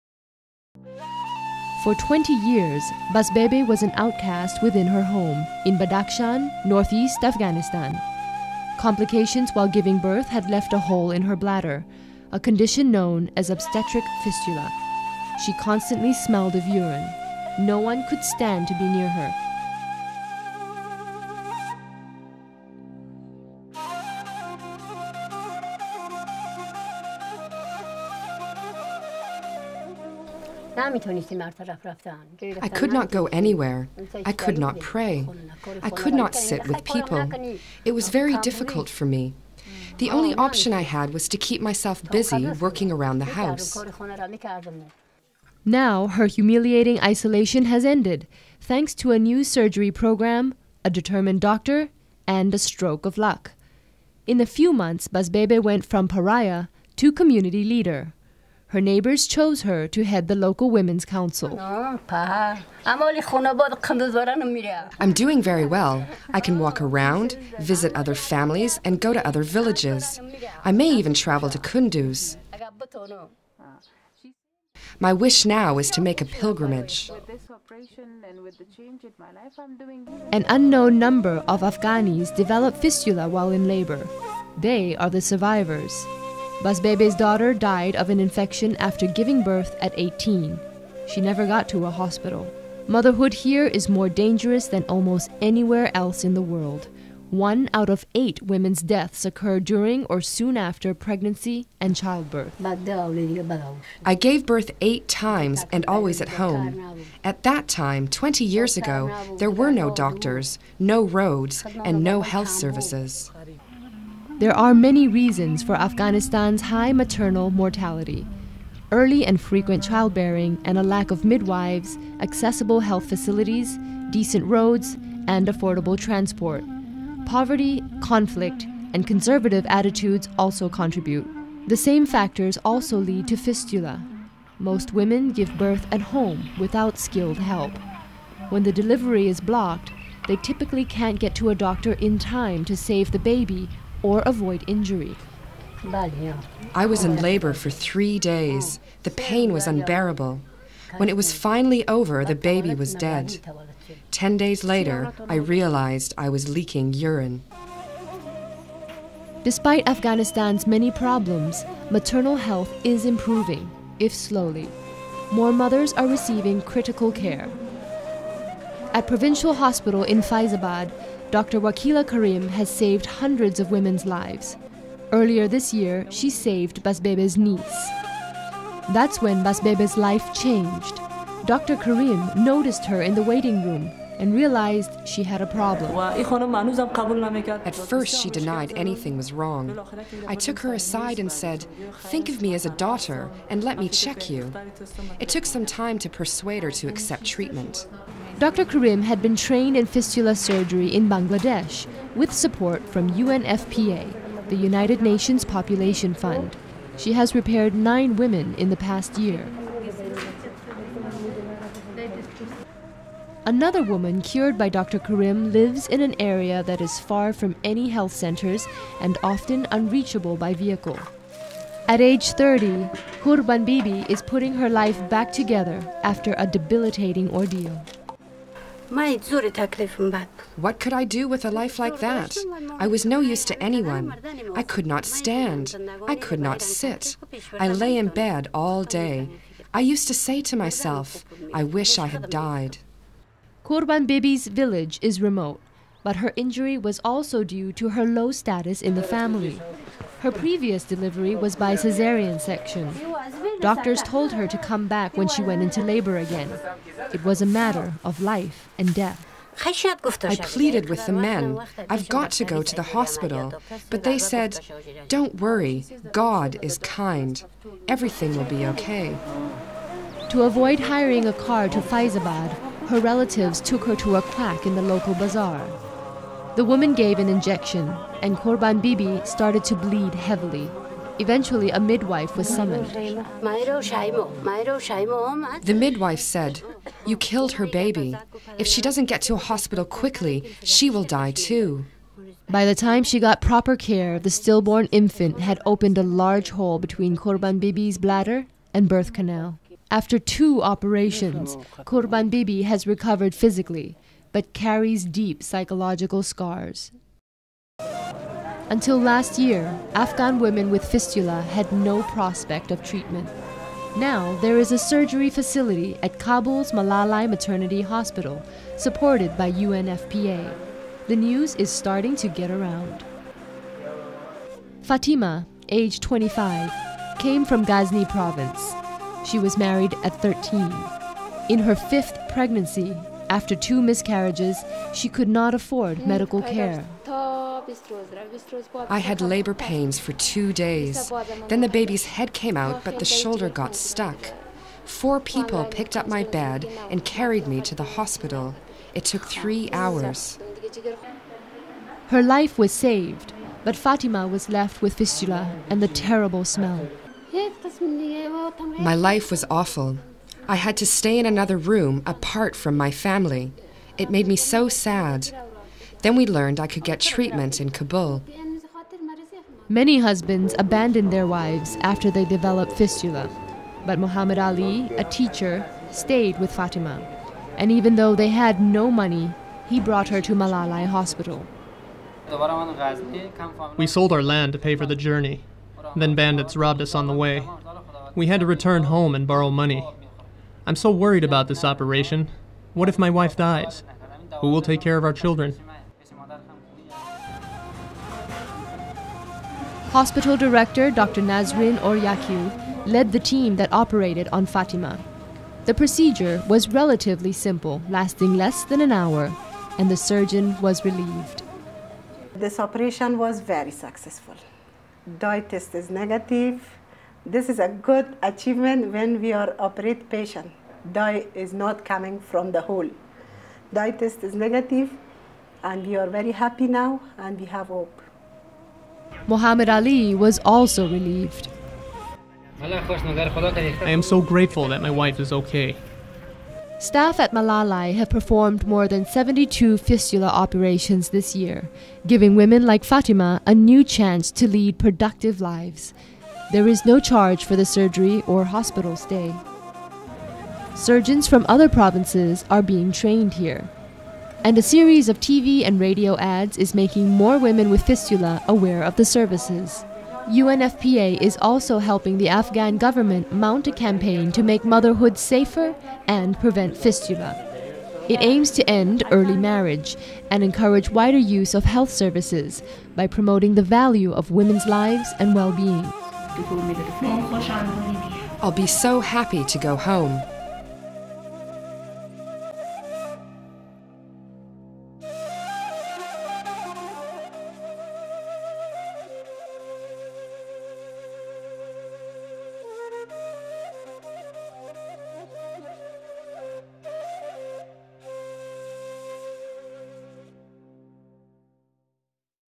Healing Fistula in Afghanistan
UNFPA has been supporting the country's first national obstetric fistula treatment facility at Malalai Maternity Hospital in Kabul. The heath center has given Afghan women who suffered with obstetric fistula a new reproductive life.